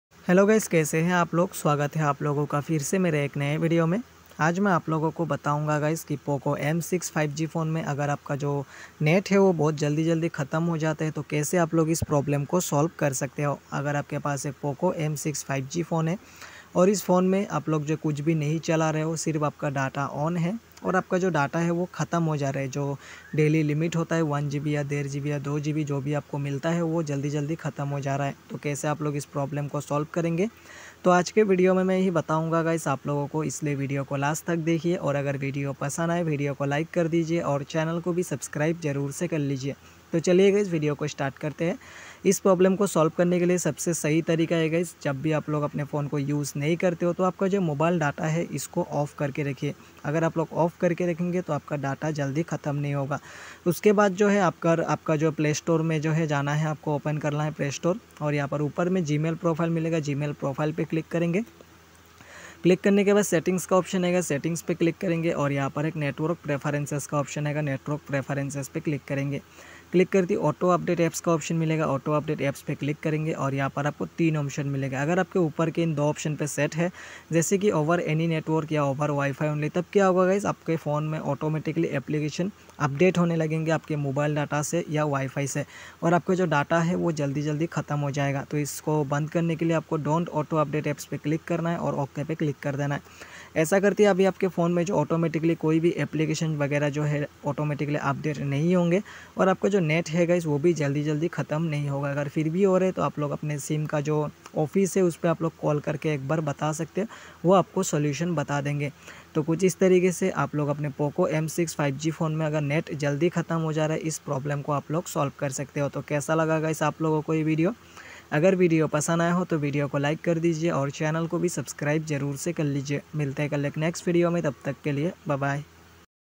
Poco M6 5g net jaldi khatam ho raha he, how to solve data ending very faster in poco, data saving in
Poco M6 5g net jaldi khatam ho raha he, how to solve data ending very faster in poco, data saving in poco, data saver settings